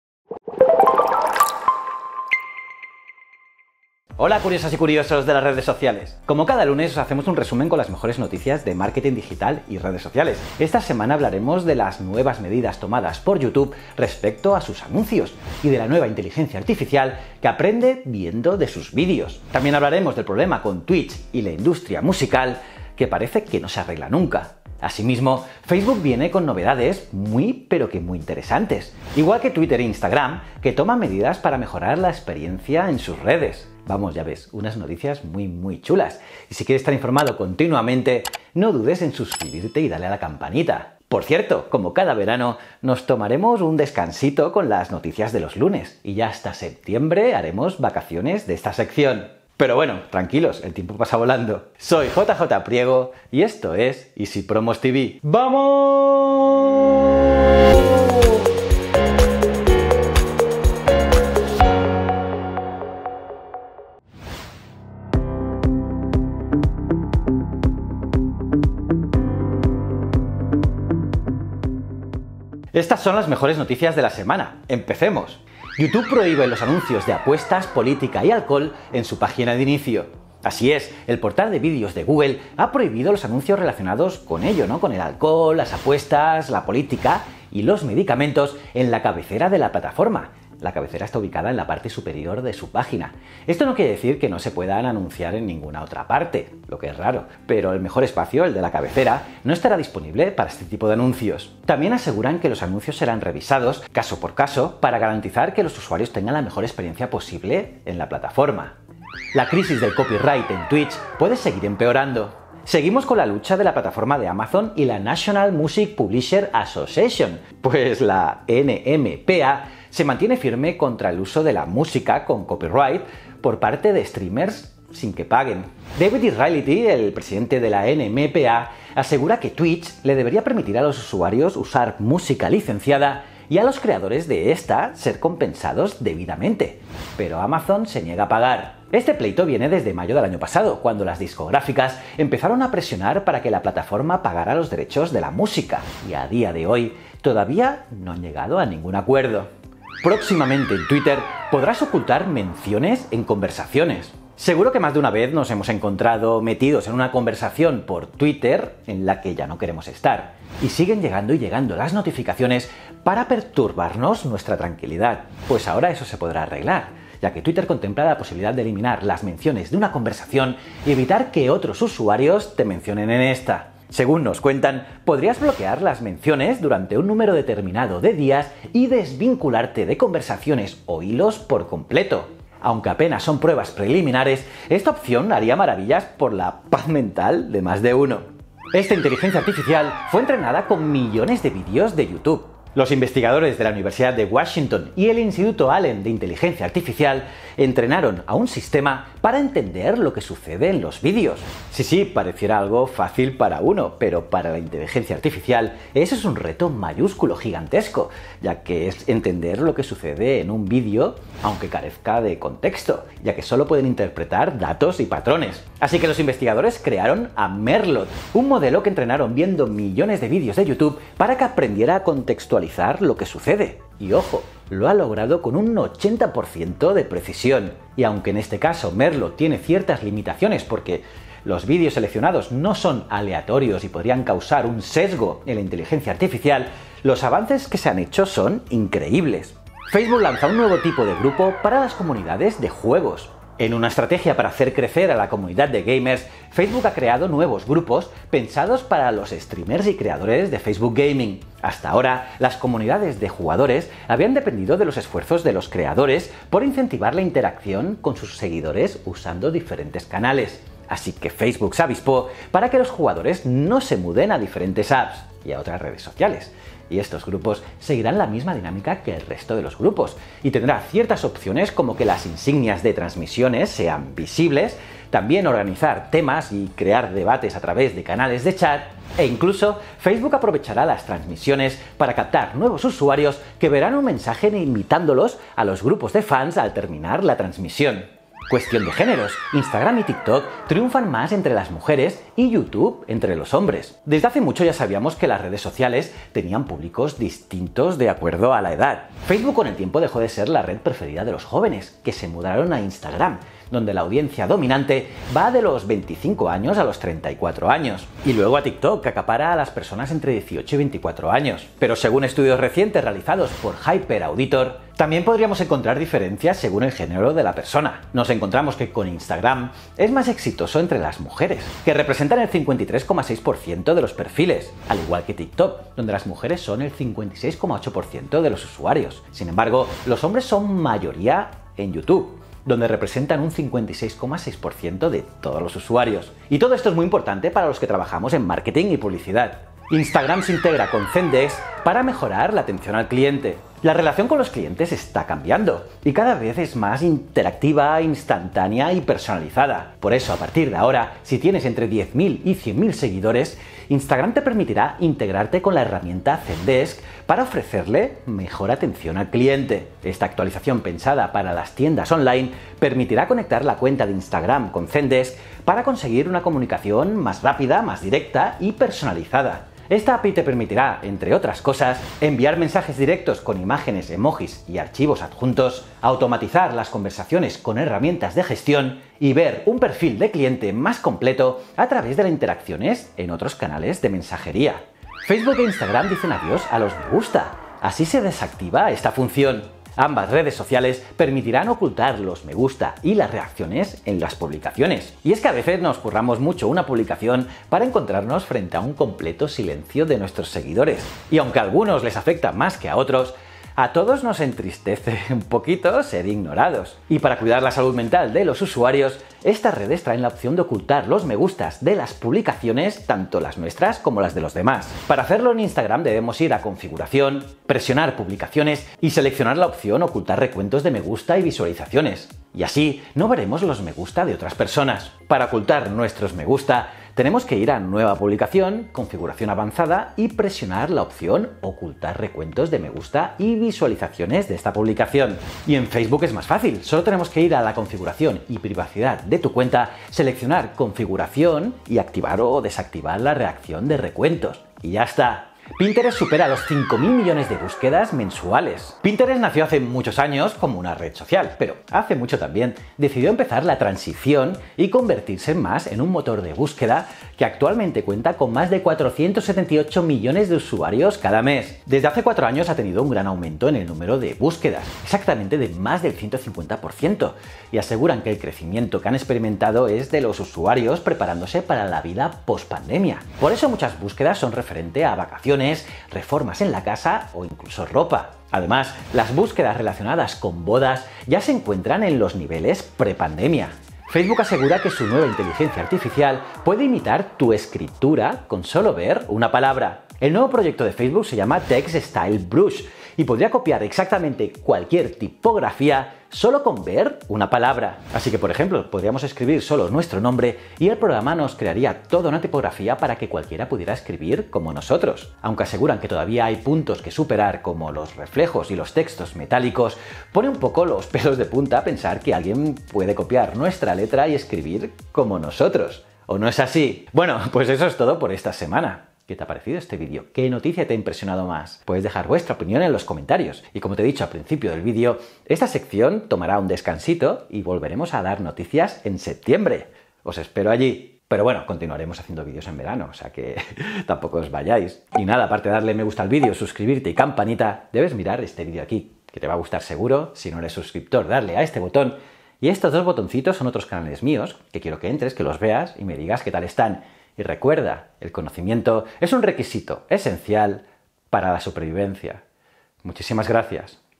🔥 Facebook e Instagram dicen adiós a los "me gusta" | Noticias Marketing y Redes Sociales
Esta semana hablaremos de las nuevas medidas tomadas por YouTube respecto a sus anuncios y de la nueva inteligencia artificial que aprende viendo sus videos, también hablaremos del problema con Twitch y la industria musical, que todavía no se resuelve.
Asimismo, Facebook viene con novedades muy pero que muy interesantes, igual que Twitter e instagram, que toman medidas para mejorar la experiencia en sus redes.

0:00 Introducción
1:16 YouTube prohíbe los anuncios de apuestas, política y alcohol en su página de inicio
1:48 La crisis del copyright en Twitch puede seguir empeorando
2:28 Próximamente en Twitter: podrás ocultar menciones en conversaciones
3:07 Esta IA fue entrenada con millones de vídeos de YouTube
3:58 Facebook lanza un nuevo tipo de grupos vpara las comunidades de juegos
4:46 Cuestión de géneros: Instagram y TikTok triunfan más entre las mujeres. Y Youtube entre los hombres
5:40 Instagram se integra con Zendesk para mejorar la atención al cliente
6:26 Facebook e Instagram dicen adiós a los "me gusta", así se desactiva la función
7:25 Pinterest supera los 5.000 millones de búsquedas mensuales
8:03 Facebook asegura que su nueva inteligencia artificial puede imitar tu escritura con sólo ver una palabra